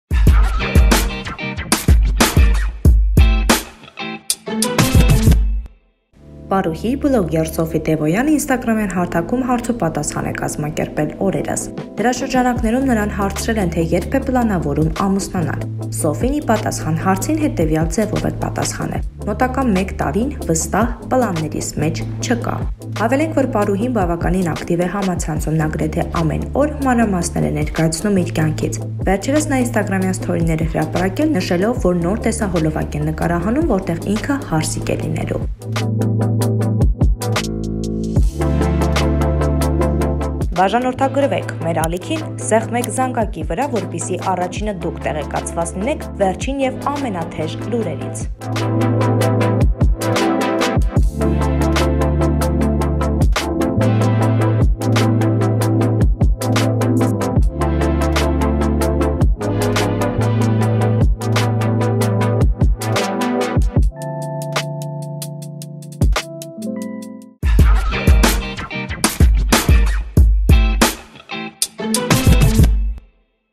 Սոֆի Դեւոյանն իր հարսանիքի մասին | Sofi Devoyan
Սոֆին վերջերս սիրելիի հետ Դուբայում էր: Նա անդադար ցույց էր տալիս, թե  ինչպես էր անցնում իրենց համատեղ հանգիստը:

Մանրամասն՝ տեսանյութում